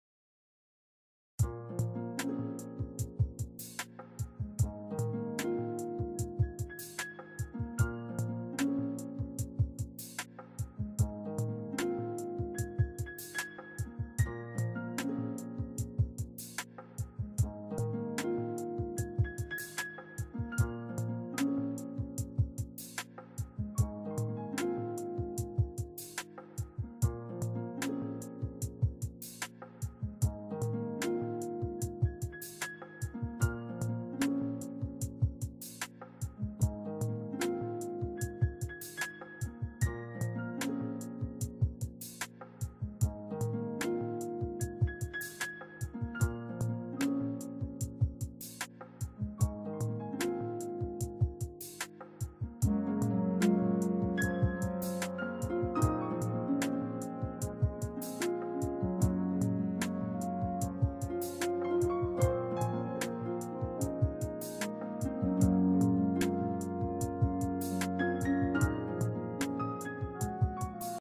solr extracting data
Extracting data from a Solr repository on Linux, just an example of what you might do. Previous movies show how environment was set up and data developed via a Nutch web crawl.